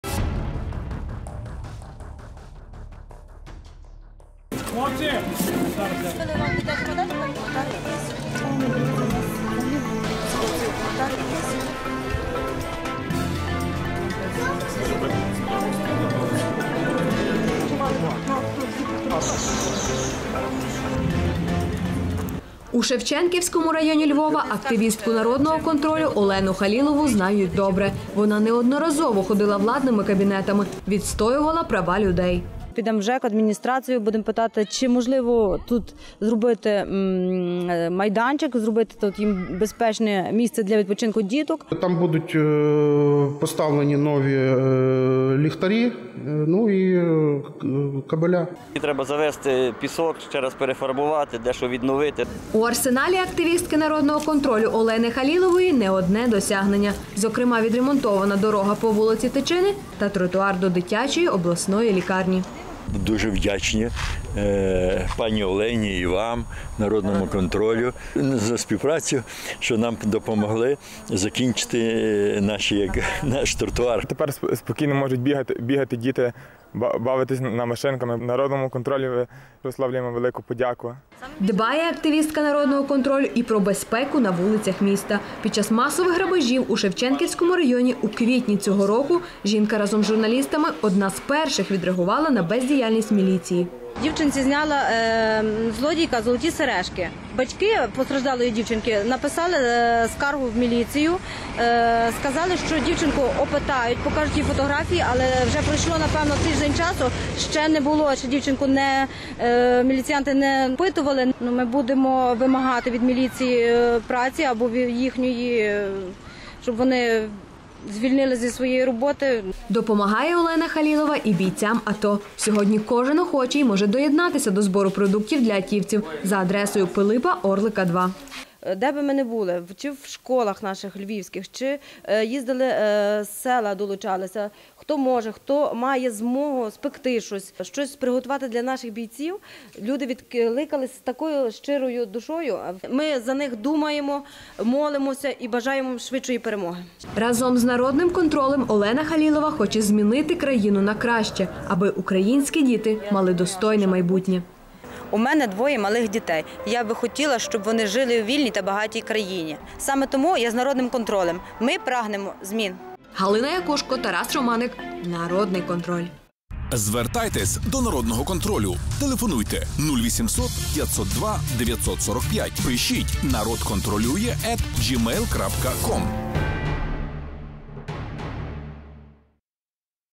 Результати роботи Олени Халілової - координатора "Народного контролю" у Шевченківському районі
Після втручання Олени Халілової відремонтовано дорогу по вулиці Тичини та тротуар до Дитячої обласної лікарні